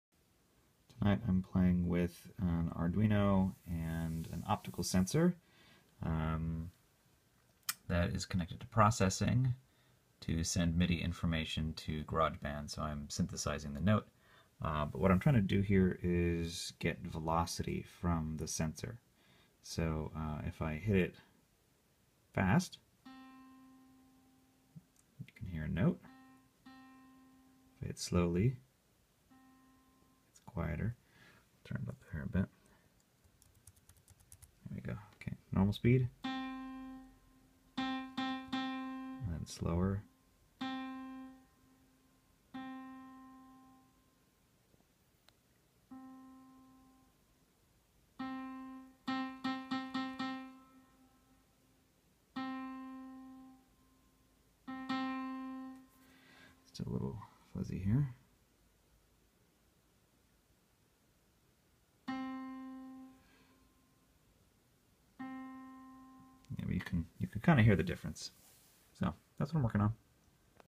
Optical Sensor Test
Using an Adruino and an optical sensor to determine MIDI velocity for an upgrade to my @mousepiano.